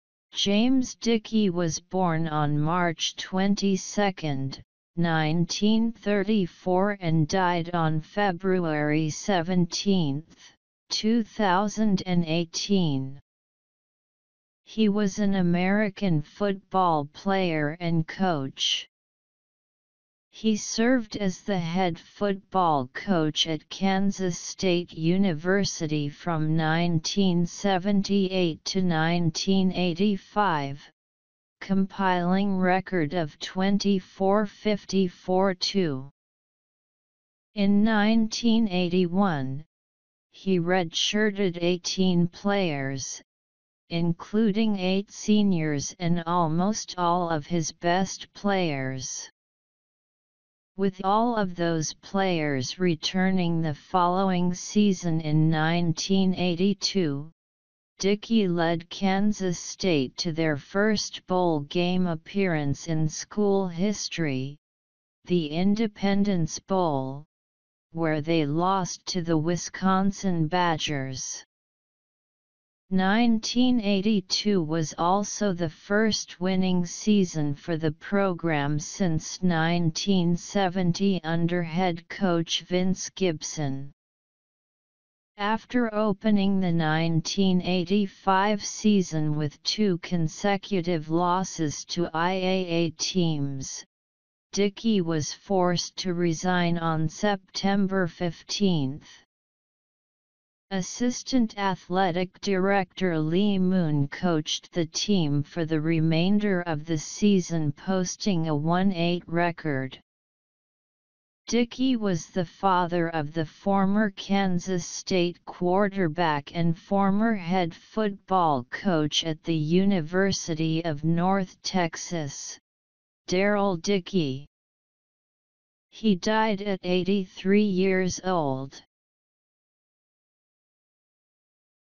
American football coach Jim Dickey Died at 83
James Dickey was born on March 22, 1934 and died on February 17, 2018. He was an American football player and coach. He served as the head football coach at Kansas State University from 1978 to 1985, compiling record of 24–54–2. In 1981, he redshirted 18 players, including eight seniors and almost all of his best players. With all of those players returning the following season in 1982, Dickey led Kansas State to their first bowl game appearance in school history, the Independence Bowl, where they lost to the Wisconsin Badgers. 1982 was also the first winning season for the program since 1970 under head coach Vince Gibson. After opening the 1985 season with two consecutive losses to I-AA teams, Dickey was forced to resign on September 15. Assistant athletic director Lee Moon coached the team for the remainder of the season posting a 1–8 record. Dickey was the father of the former Kansas State quarterback and former head football coach at the University of North Texas, Darrell Dickey. He died at 83 years old.